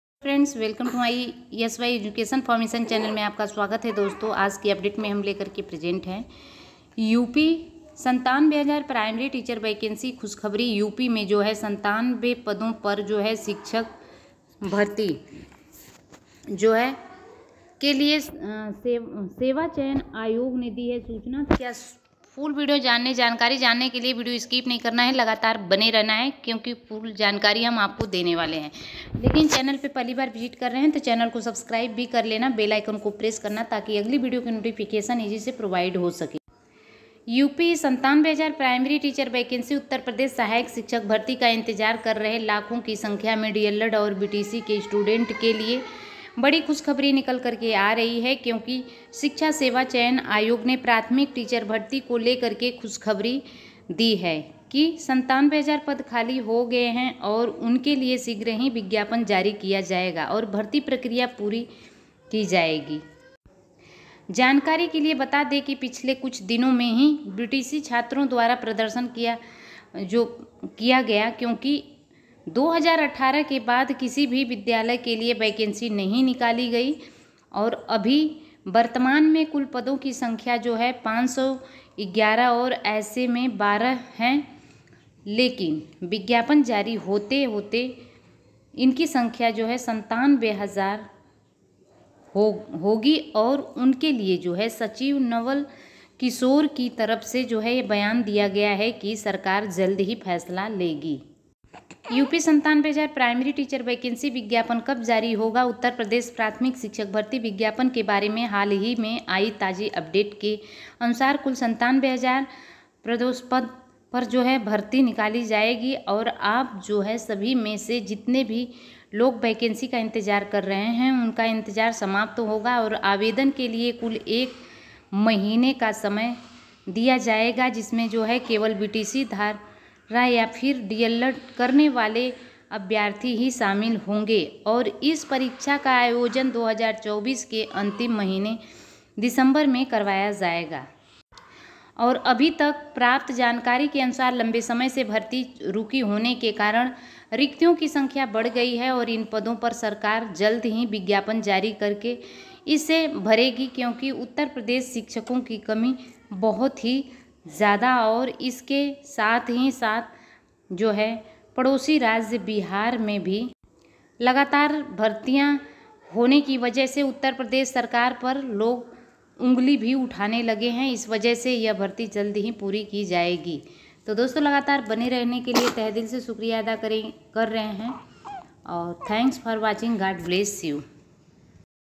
UP 97000 Primary Teacher Vacancy : खुशखबरी ! यूपी मे 97000 पदों पर शिक्षक भर्ती के लिए सेवा चयन आयोग
UP 97000 Primary Teacher Vacancy : खुशखबरी ! यूपी मे 97000 पदों पर शिक्षक भर्ती के लिए सेवा चयन आयोग ने दी सूचना